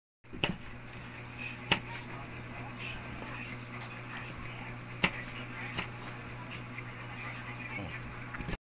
chopping eggplant
chopping eggplant creating asmr sounds